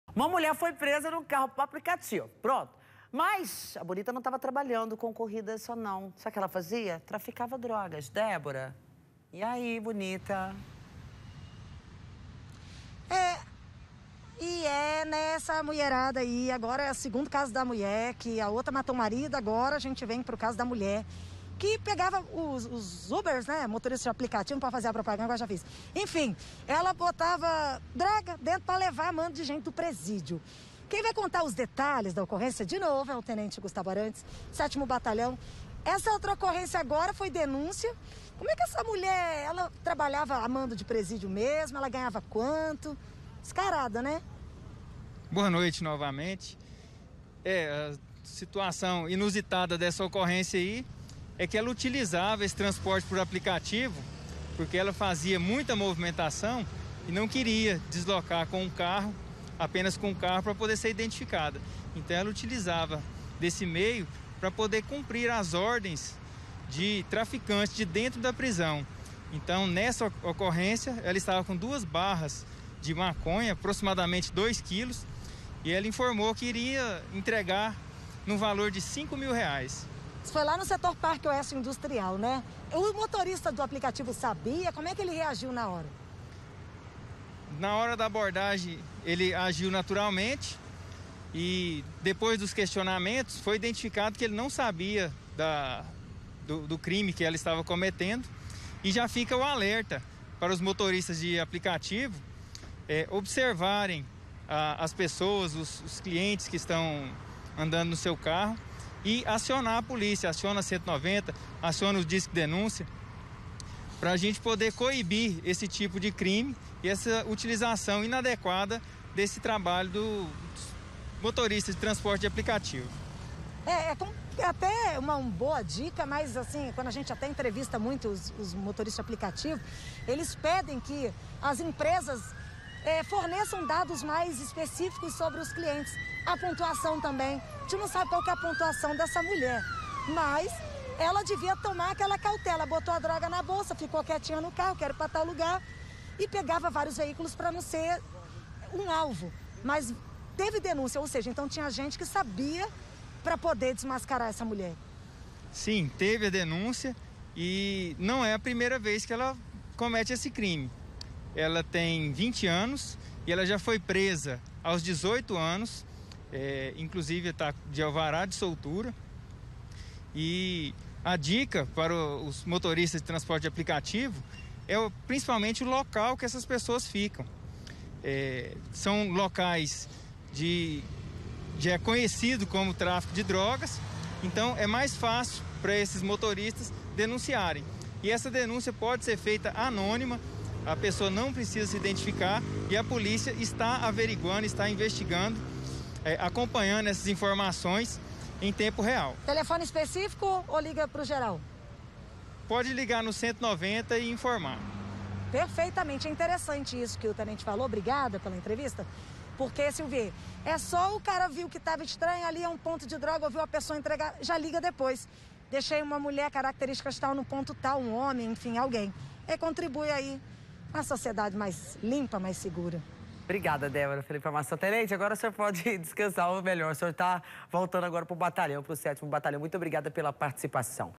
MULHER É SUSPEITA DE USAR CARRO DE APLICATIVO PARA VENDER DROGAS
UMA MULHER FOI PRESA NUM CARRO DE APLICATIVO. MAS A BONITA NÃO ESTAVA TRABALHANDO COM CORRIDAS NÃO, ELA ESTAVA TRAFICANDO.